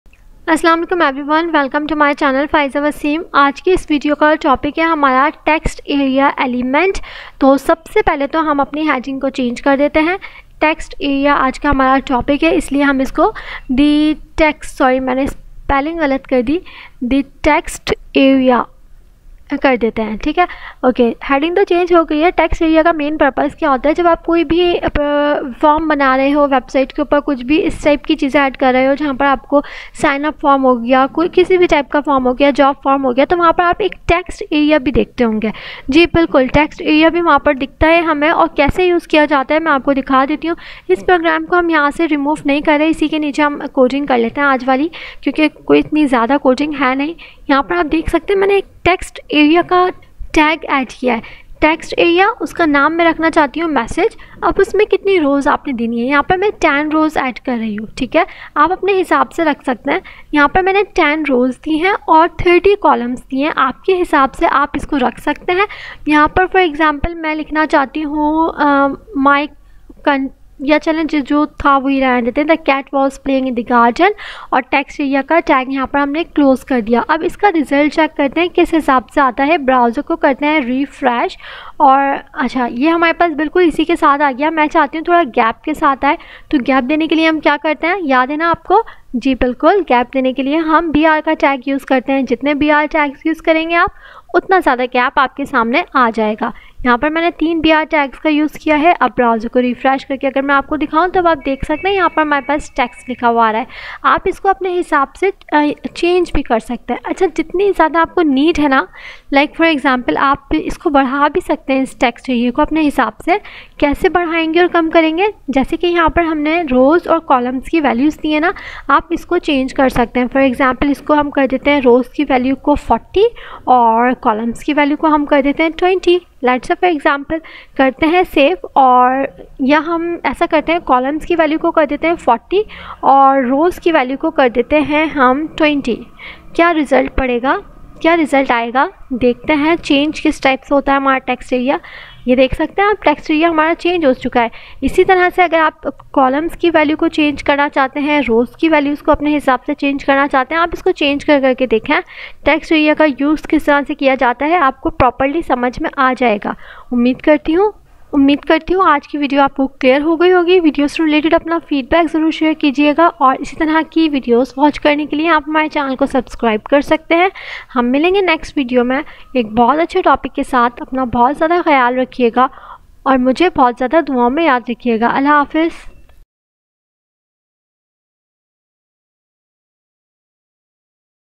Web designing ! Lec 74 HTML TextArea Element ! Textarea tag in html
Web designing ! Lec 74 HTML TextArea Element ! Textarea tag in html HTML multi-line text input Web designing full course in urdu step by step
How to create text area in html?
How to create multi line input box in html?
When to create text area in html?